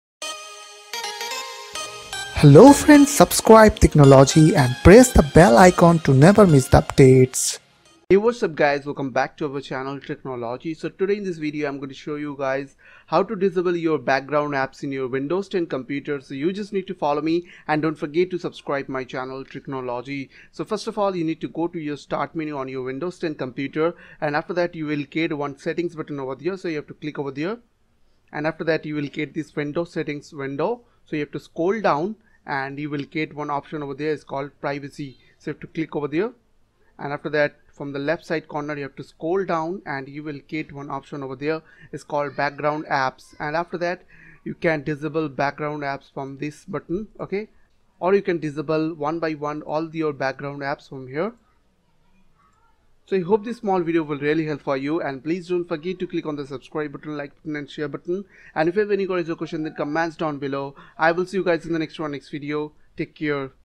How To Disable All Background Apps in Windows 10 With One Click
In this Video, I will show you guys how to Turn-off or Turn-On background apps from windows 10 pc or laptops.

1. Mic
2. Microphone Stand
3. Headphone
4. Phone
5. Strip LED Light
6. Camera